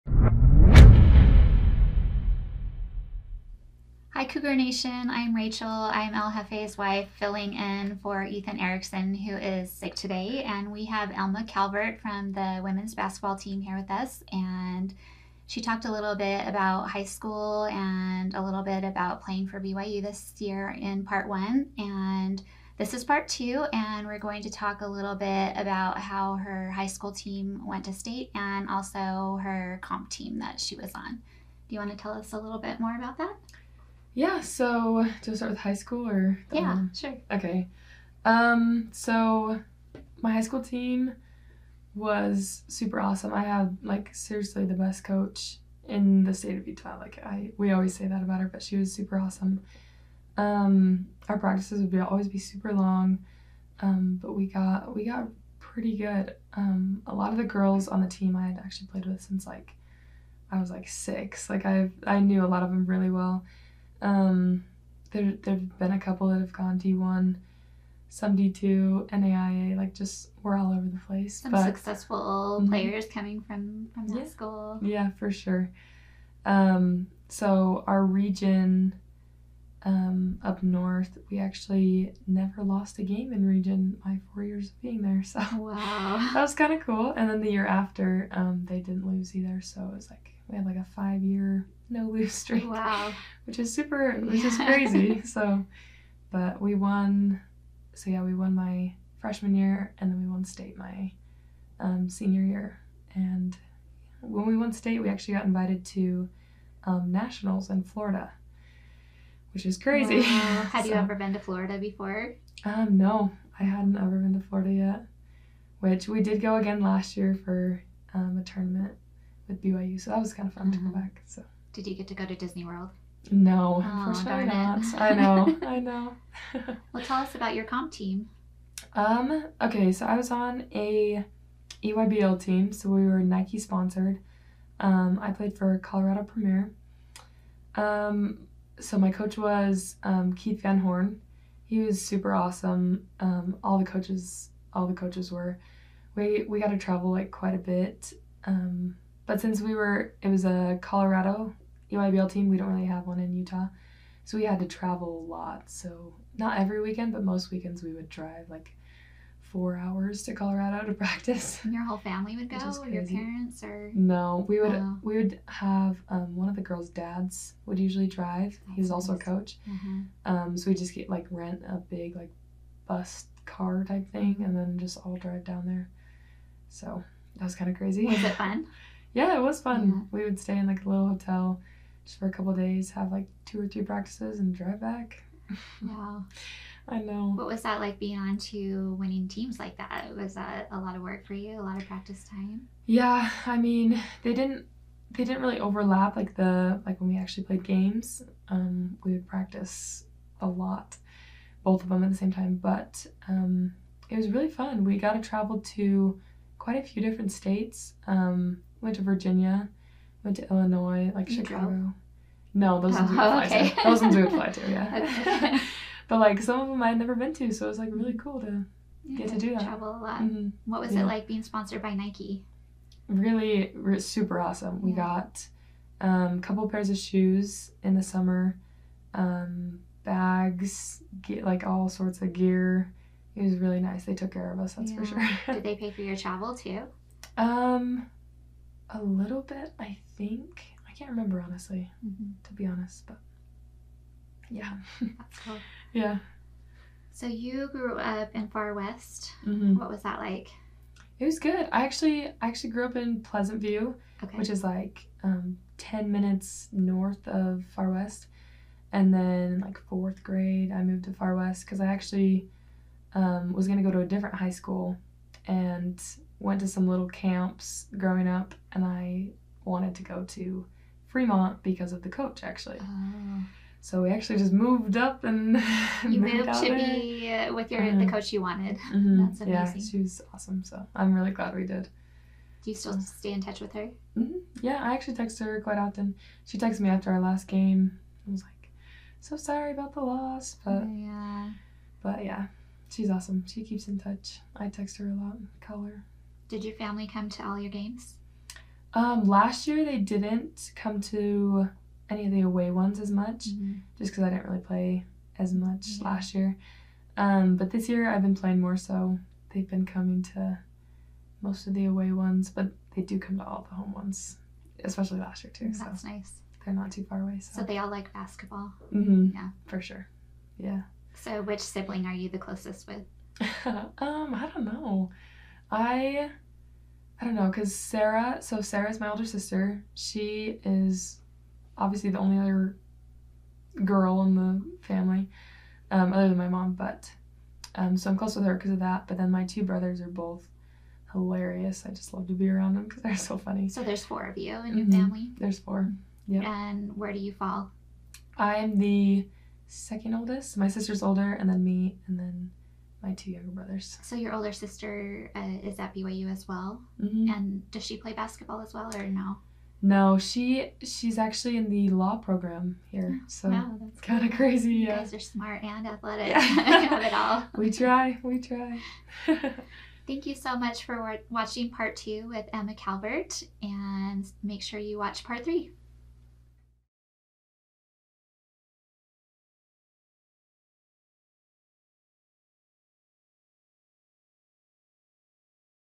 Emma Calvert BYU forward-High school basketball and competitive basketball
Emma Calvert shares about her high school basketball experience, playing competitive basketball, and her relationship with her siblings.